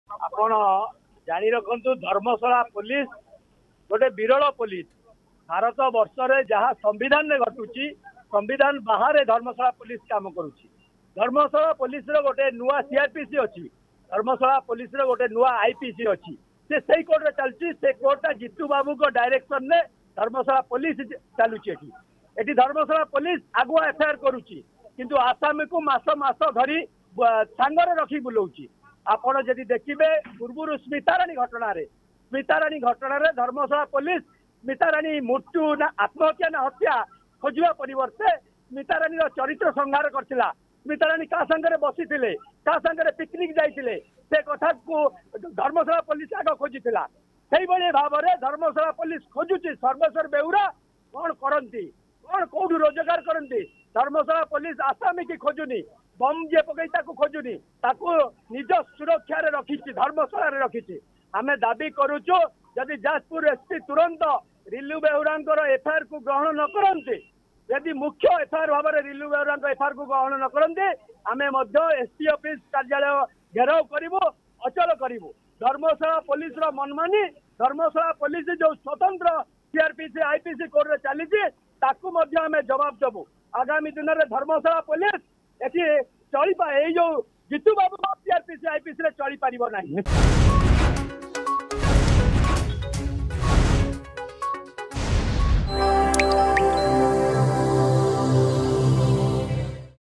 ସମ୍ବିଧାନ ବାହାରେ କାର୍ଯ୍ୟ କରୁଛି ଧର୍ମଶାଳା ପୋଲିସ୍
ଧର୍ମଶାଳା ପୋଲିସ ଅଭିଯୁକ୍ତମାନଙ୍କୁ ସୁରକ୍ଷା ଦେଉଛି। ଧର୍ମଶାଳା ପୋଲିସର ମନମୁଖୀ ଶାସନ ଚଳିବ ନାହିଁ...